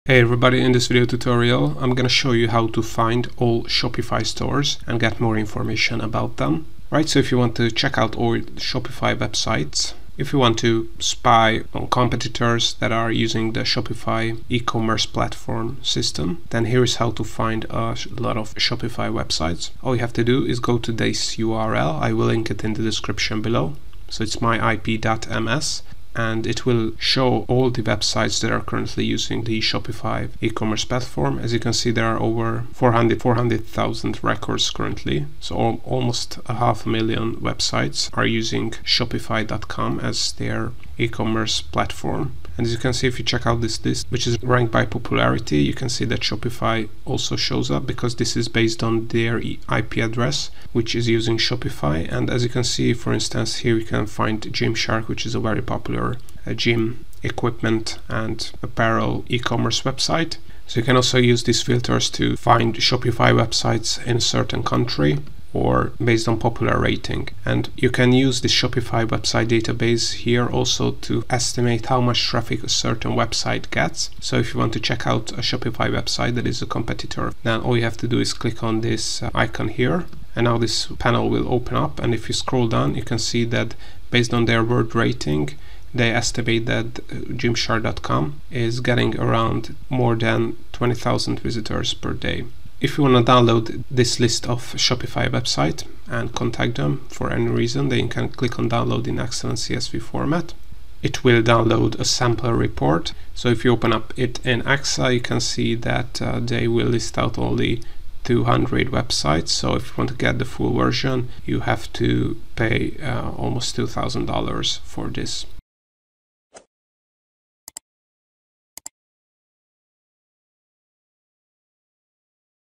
How To Find All Shopify Stores? [in 2025] (Spy On Your Shopify Competitors)
. - Learn how to find all Shopify stores that are using Shopify as their e-commerce platform.

𝐅𝐑𝐄𝐄 𝐀𝐮𝐝𝐢𝐨𝐛𝐨𝐨𝐤 𝐖𝐢𝐭𝐡 𝐀𝐮𝐝𝐢𝐛𝐥𝐞